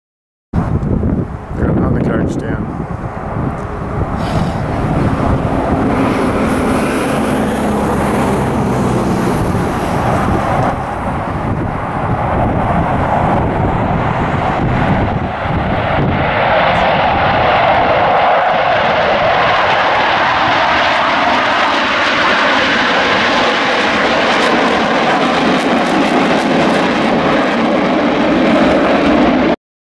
RAAF F/A-18 Hornets Four Ship "Dirty" Pass
Four ship F/A-18s formation with gear/hooks down